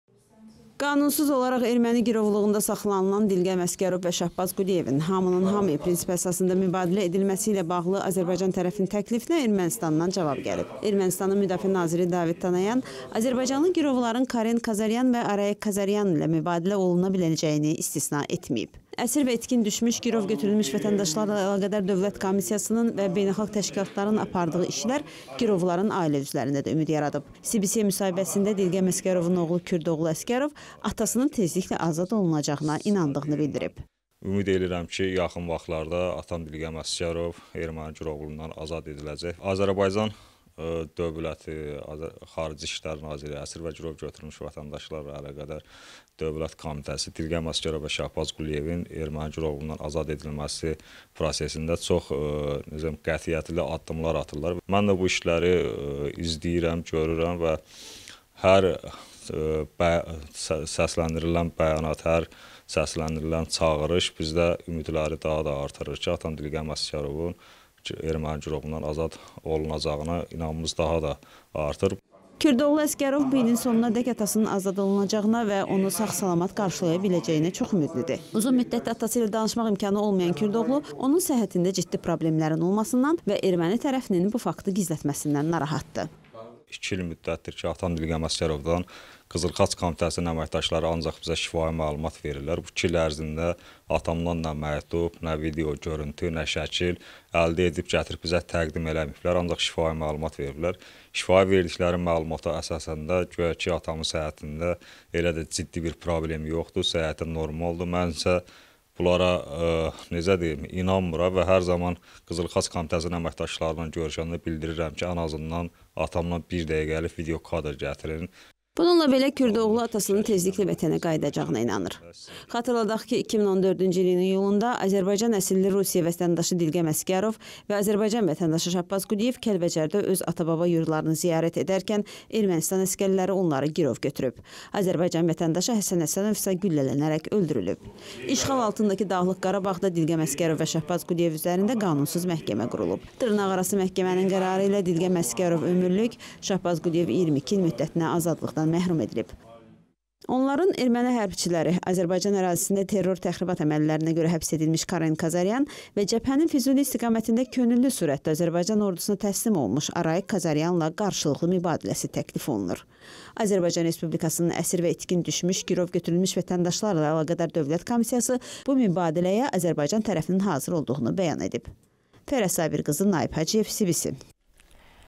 DİLQƏM ƏSGƏROVUN OĞLU ÜMİDLİDİR
Qanunsuz olaraq erməni girovluğunda saxlanılan Dilqəm Əsgərov və Şahbaz Quliyevin “hamının-hamıya” prinsipi əsasında mübadilə edilməsi ilə bağlı Azərbaycan tərəfinin təklifinə Ermənistandan cavab gəlib. Ermənistanın müdafiə naziri David Tonoyan azərbaycanlı girovların Karen Kazaryan və Araik Kazaryan ilə mübadilə oluna biləcəyini istisna etməyib. Əsir və itkin düşmüş, girov götürülmüş vətəndaşlarla əlaqədar Dövlət Komissiyasının və beynəlxalq təşkilatların apardığı işlər girovların ailə üzvlərində də ümid yaradıb. CBC-yə müsahibəsində Dilqəm Əsgərovun oğlu Kürdoğlu Əsgərov atasının tezliklə azad olunacağına inandığını bildirib.
Kürdoğlu Əsgərov bu ilin sonunadək atasının azad olunacağına və onu sağ-salamat qarşılaya biləcəyinə çox ümidlidir. Uzun müddətdir atası ilə danışmaq imkanı olmayan Kürdoğlu onun səhhətində ciddi problemlərin olmasından və erməni tərəfinin bu faktı gizlətməsindən narahatdır. Bununla belə, Kürdoğlu atasının tezliklə vətənə qayıdacağına inanır.
Xatırladaq ki, 2014-cü ilin iyulunda Azərbaycan əsilli Rusiya vətəndaşı Dilqəm Əsgərov və Azərbaycan vətəndaşı Şahbaz Quliyev Kəlbəcərdə öz ata-baba yurdlarını ziyarət edərkən Ermənistan əsgərləri onları girov götürüb, Azərbaycan vətəndaşı Həsən Həsənov isə güllələnərək öldürülüb. İşğal altındakı Dağlıq Qarabağda Dilqəm Əsgərov və Şahbaz Quliyev üzərində qanunsuz "məhkəmə" qurulub. "Məhkəmə"nin qərarı ilə D.Əsgərov ömürlük, Ş.Quliyev 22 il müddətinə azadlıqdan məhrum edilib. Onların erməni hərbçiləri - Azərbaycan ərazisində terror-təxribat əməllərinə görə həbs edilmiş Karen Kazaryan və cəbhənin Füzuli istiqamətində könüllü surətdə Azərbaycan Ordusuna təslim olmuş Araik Kazaryanla qarşılıqlı mübadiləsi təklif olunur. Azərbaycan Respublikasının Əsir və itkin düşmüş, girov götürülmüş vətəndaşlarla əlaqədar Dövlət Komissiyası bu mübadiləyə Azərbaycan tərəfinin hazır olduğunu bəyan edib.